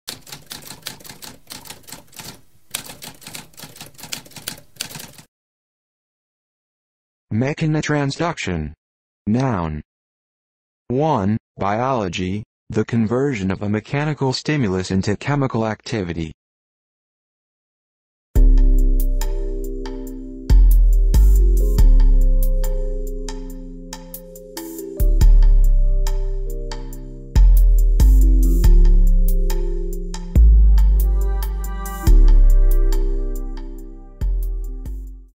What does mechanotransduction mean?
A spoken definition of mechanotransduction.

Intro Sound:
Typewriter - Tamskp